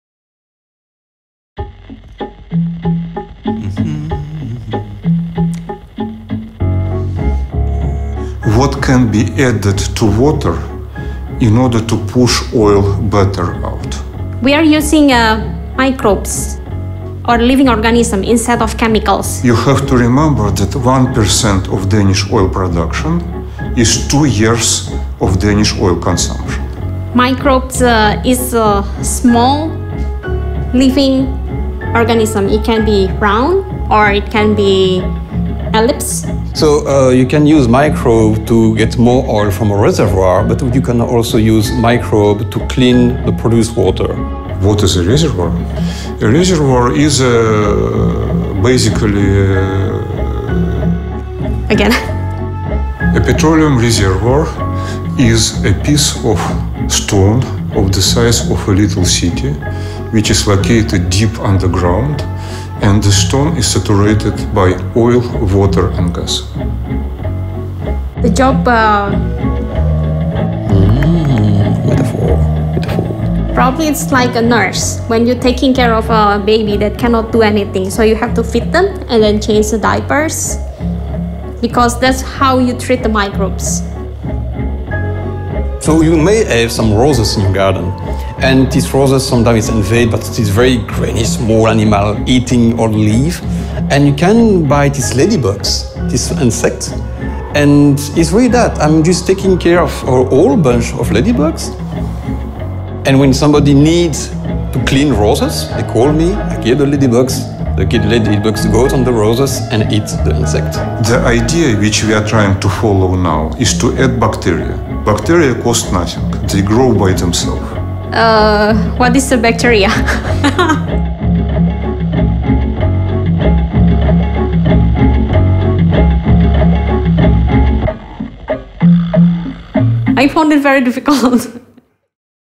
Maersk and the Danes – BioRec and Green Tools in Oil Recovery
A large scale Danish research program, BioRec, investigates how microbes and enzymes may be utilized to enhance oil recovery and prevent corrosion and formation of gas hydrates during exploration.